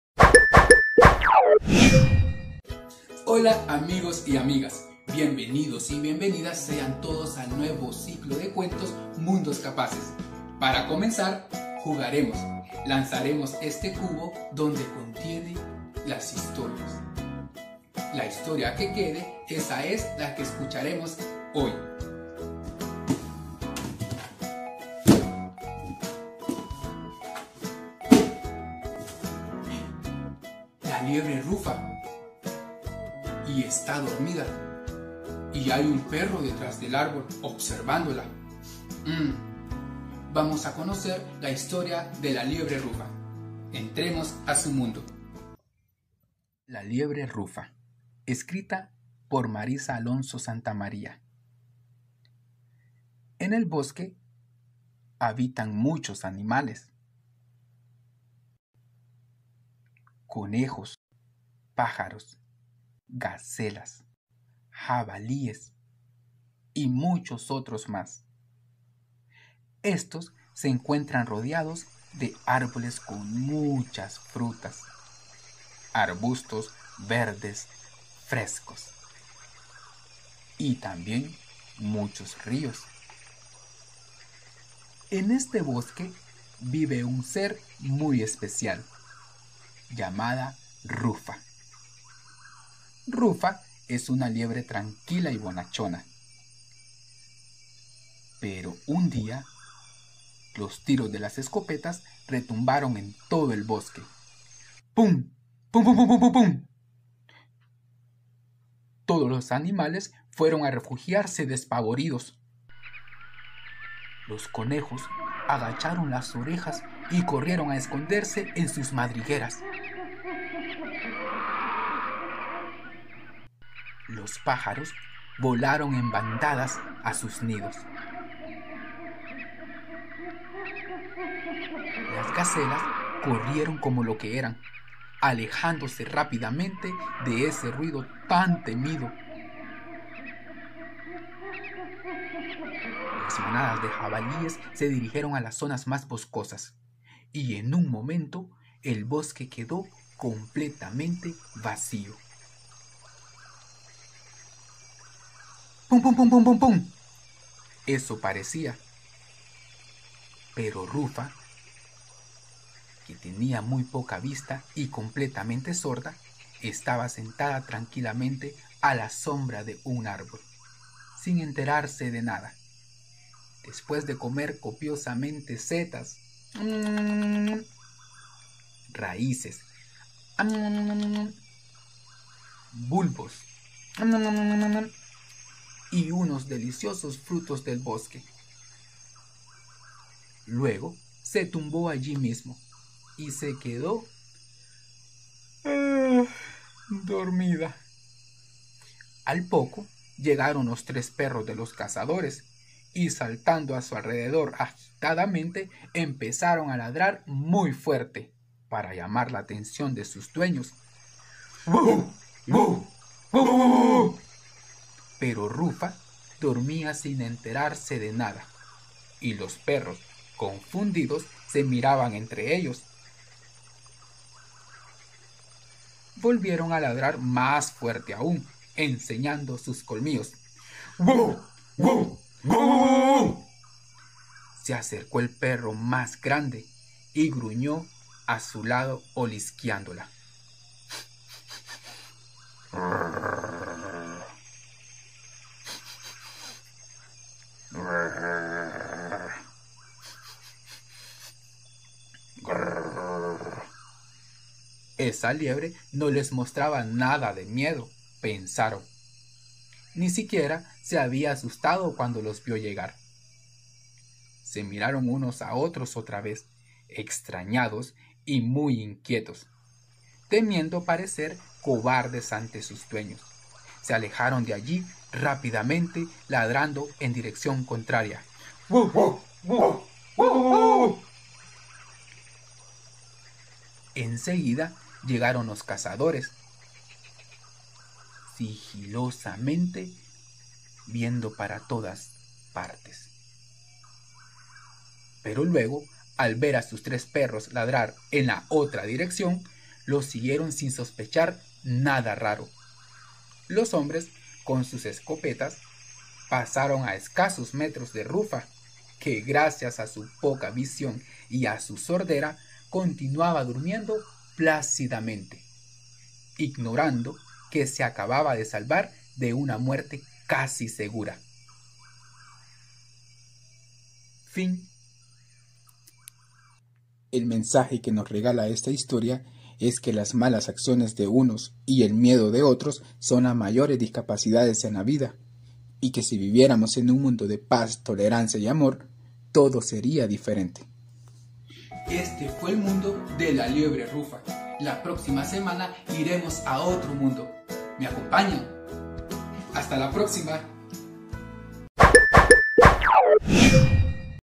Muchos cuentos por leer: La liebre Rufa
Una liebre tranquila y bonachona, pero, tenía una discapacidad, era muy corta de vista y completamente sorda.
Escrita por Marisa Alonso Santamaria, una adaptación por Kevín Ávila.
ODS10 Reducción de las desigualdades